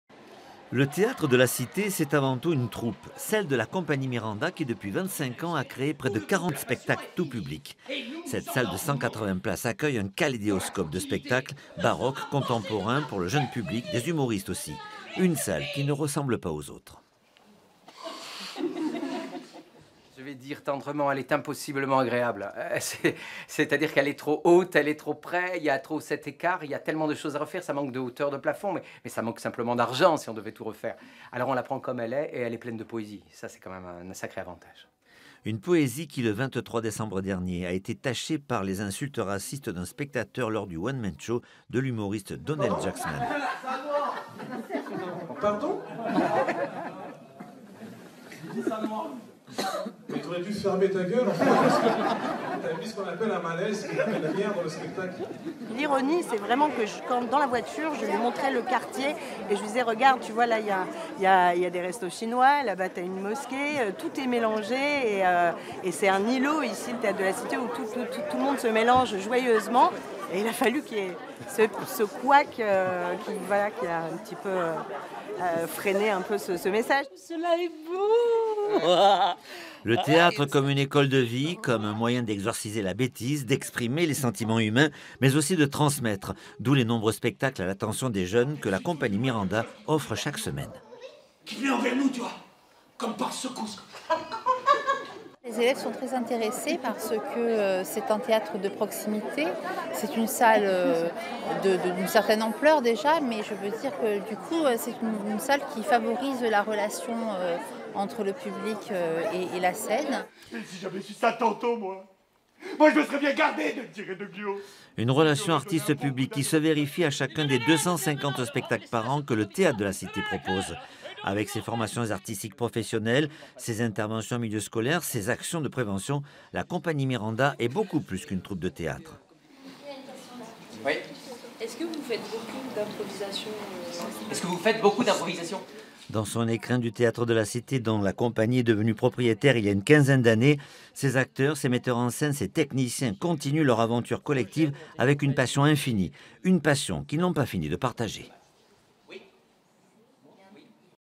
Nice : la compagnie Miranda et le théâtre de la cité
En plein de Nice, le théâtre de la cité. Il appartient à l’une des plus anciennes compagnies de la région, la compagnie Miranda. Depuis 25 ans, elle a créé plus de 40 spectacles, baroques, humoristiques ou contemporains. Il y a pour tous les goûts !

Intervenants :
- Thierry Surace
Directeur artistique Théâtre de la Cité
- Sylvia Scantamburlo
Comité de programmation Théâtre de la Cité
- Florence Hyjazi
Professeur de lettres Lycée Calmette Nice

Equipe :
B. Persia, P.O Casabianca, A. Taba